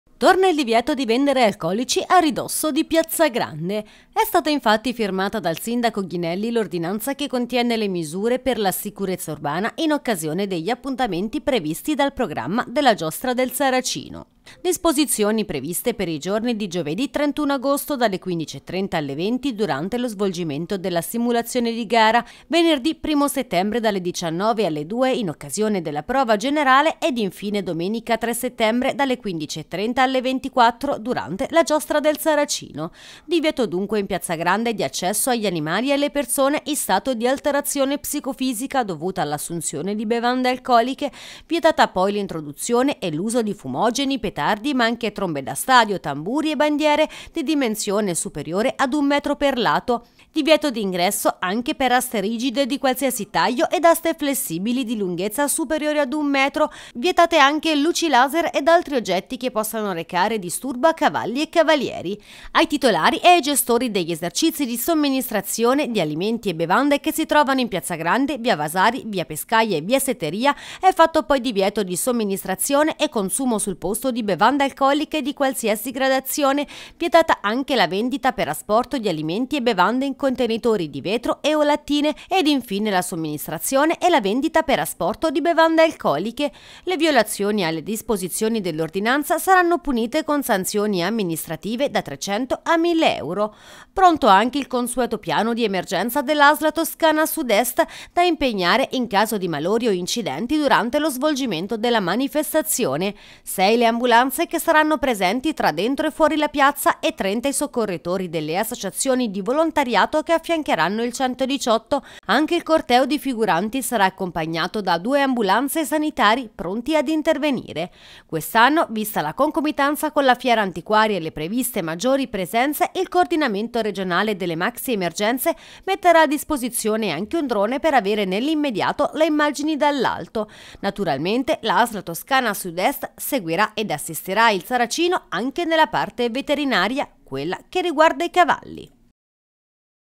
Giostra, le misure di sicurezza in piazza Grande. Pronto anche il piano della Asl
Torna il divieto di vendere alcolici a ridosso di piazza Grande.
E' stata infatti firmata dal sindaco Ghinelli l'ordinanza che contiene le misure per la sicurezza urbana in occasione degli appuntamenti previsti dal programma della Giostra del Saracino.
Disposizioni previste per i giorni di giovedì 31 agosto dalle ore 15:30 alle ore 20:00 durante lo svolgimento della “Simulazione di Gara”, venerdì 1 settembre dalle ore 19:00 alle ore 2:00 in occasione della “Prova Generale”, domenica 3 settembre dalle ore 15:30 alle ore 24:00 durante la “Giostra del Saracino”.
Divieto di ingresso in piazza Grande agli animali ed alle persone in stato di alterazione psicofisica dovuta all'assunzione di bevande alcoliche. Vietata poi l'introduzione e uso di fumogeni, petardi ma anche, trombe da stadio, tamburi, bandiere (di dimensione superiore ad un metro per lato), aste rigide di qualsiasi taglio ed aste flessibili di lunghezza superiore ad un metro, luci laser e altri oggetti atti ad arrecare disturbo a cavalli e cavalieri.
Ai titolari ed ai gestori degli esercizi di somministrazione di alimenti e bevande che si trovano in piazza Grande, via Vasari, via Pescaia e via Seteria, è fatto divieto di somministrazione e consumo sul posto di bevande alcoliche di qualsiasi gradazione; vietata inoltre la vendita per asporto di alimenti e bevande in contenitori di vetro e/o in lattine e la somministrazione e la vendita per asporto di bevande alcoliche.
Le violazioni alle disposizioni dell'ordinanza sono punite con sanzioni amministrative da € 300,00 a € 1.000,00.
Pronto anche il consueto piano di emergenza della Asl Toscana Sud Est, da impiegare in caso di malori ed incidenti durante lo svolgimento della manifestazione.
Sei le ambulanze che saranno presenti tra dentro e fuori la piazza e 30 i soccorritori delle associazioni di volontariato che affiancheranno il 118. Anche il corteo di figuranti sarà accompagnato da due ambulanze e sanitari, pronti ad intervenire.
Quest'anno, vista la concomitanza con la fiera antiquarie e le previste maggiori presenze, il coordinamento regionale delle maxi emergenze metterà a disposizione un drone per avere nell’immediato le immagini dall’alto.
Naturalmente la Asl Toscana sud est seguirà ed assisterà il Saracino anche nella parte veterinaria, quella riguardante i cavalli.